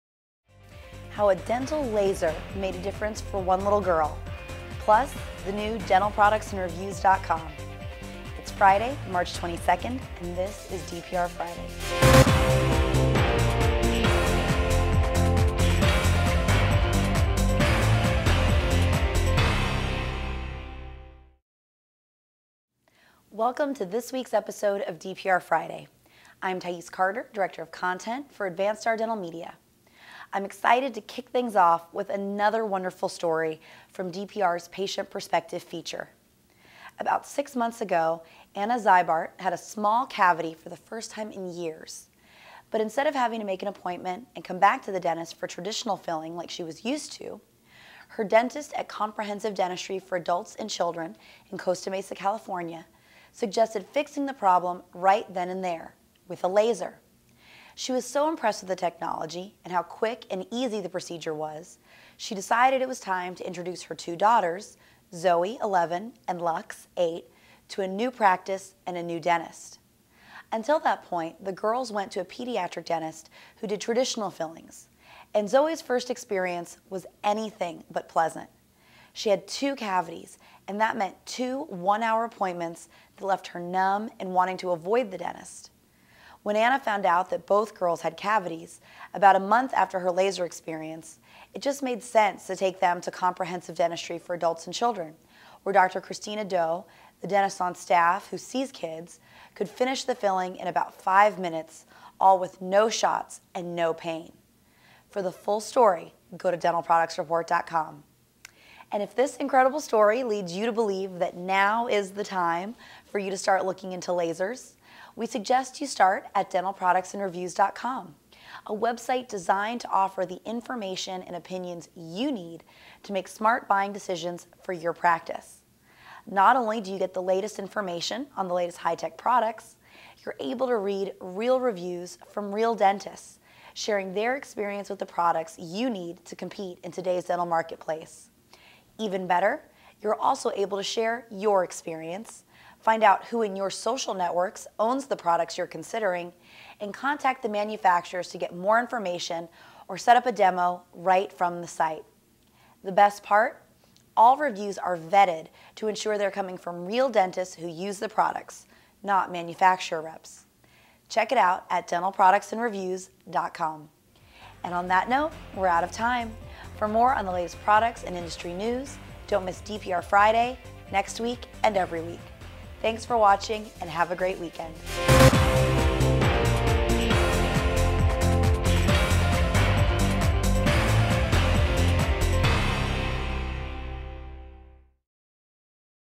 Dental Laser from BIOLASE is Changing the Perspective of the Dental Visits
WaterLase is a safe and gentle alternative to traditional dental tools. Using laser and water spray, WaterLase can perform many procedures without a shot or a drill. Finally, going to the dentist is fun again!